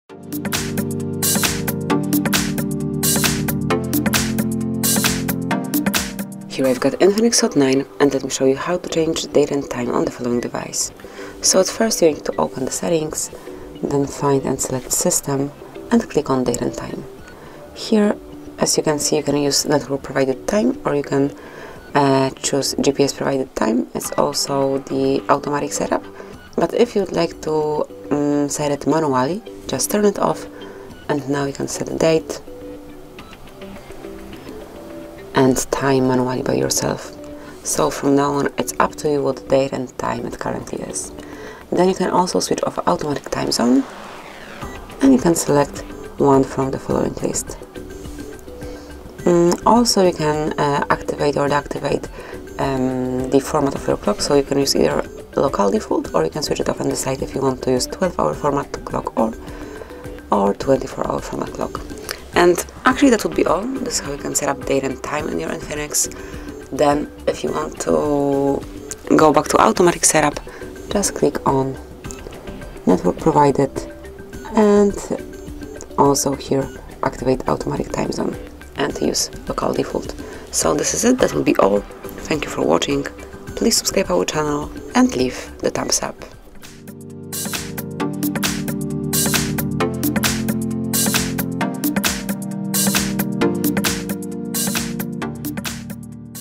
How to Change Date and Time in INFINIX Hot 9 – Open Date and Time Settings
Learn more info about INFINIX Hot 9:
Do you want to set the date and time on your INFINIX Hot 9, but cannot find the correct settings? Don't worry because we are here for you and we will show you how to do it efficiently. Follow our instructions and easily set up both - time and date on your INFINIX device. Also, pick the automatic settings or do it manually if you want. Choose the time zone and select a clock format.